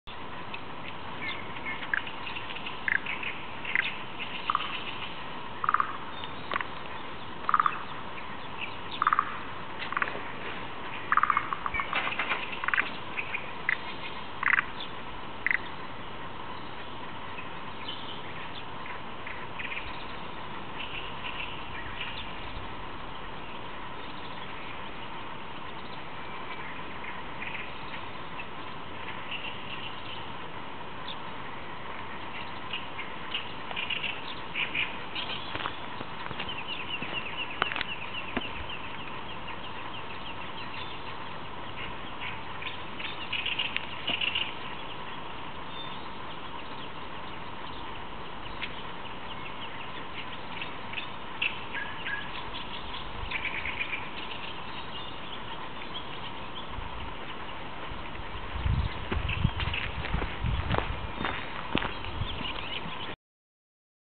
チゴモズの羽繕いです。飛び去ります。